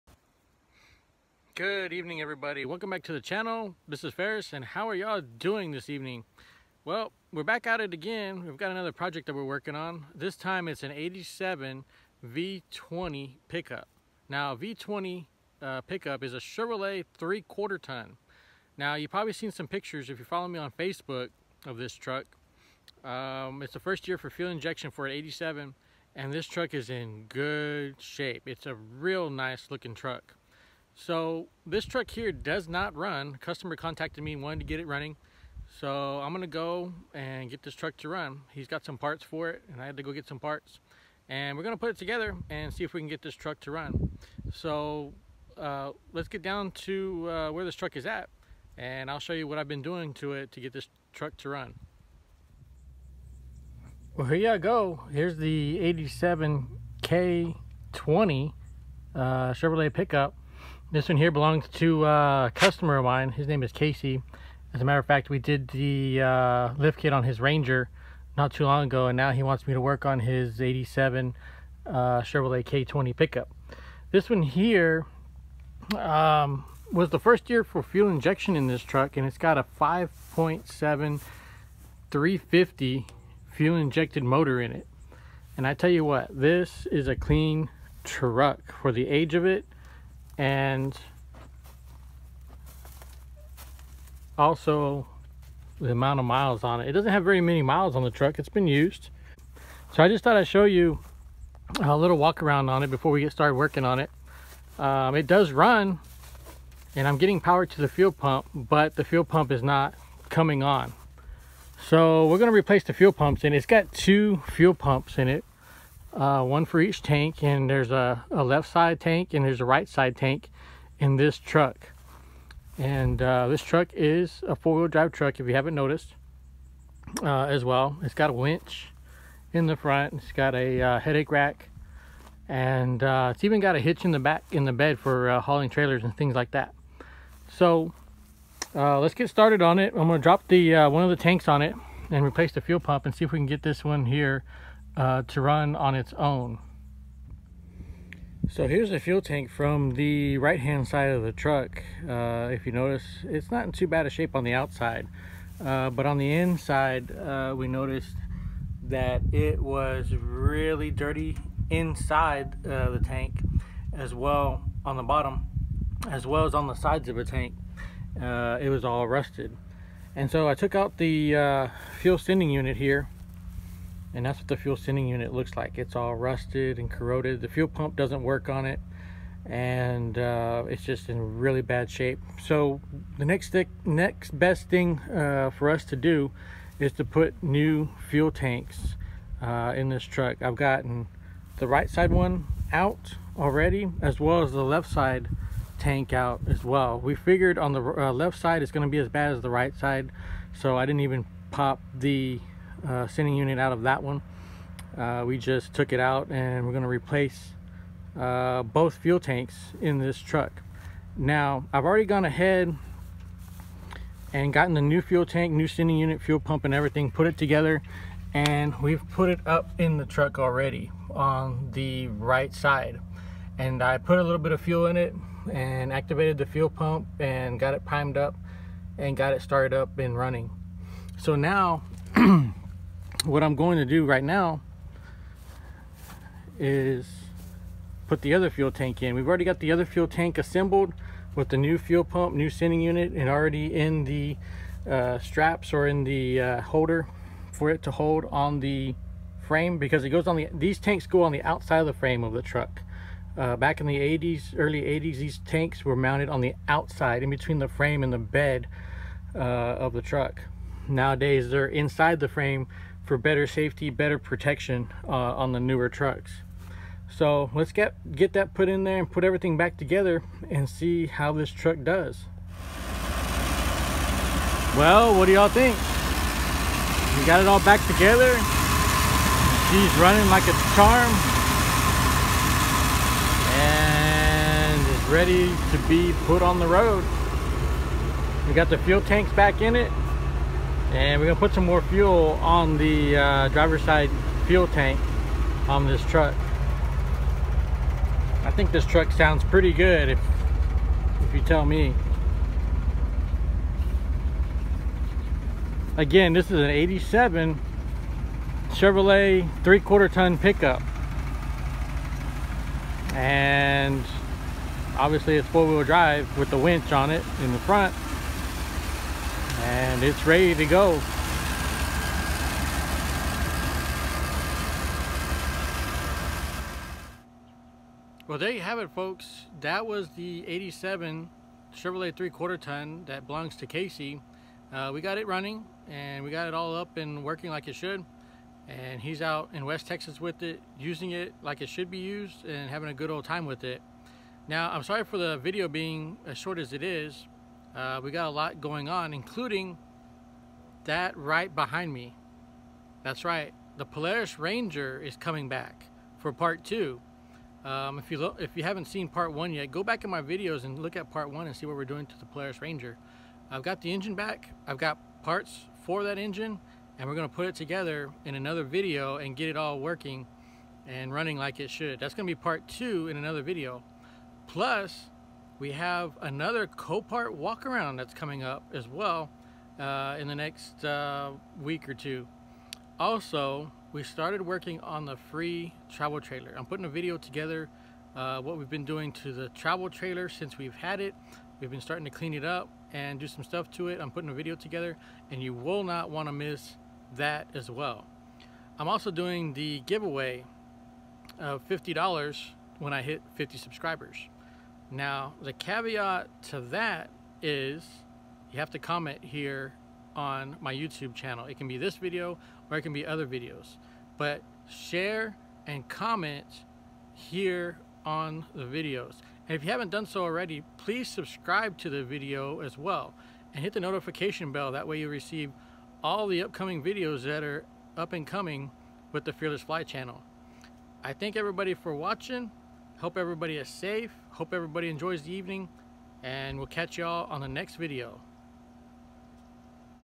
Getting an 87 Chevrolet 3/4 ton to run
In this video a customer called me back after doing some previous work to get his 87 Chevrolet 3/4 ton running. check out what I had to do to get it running.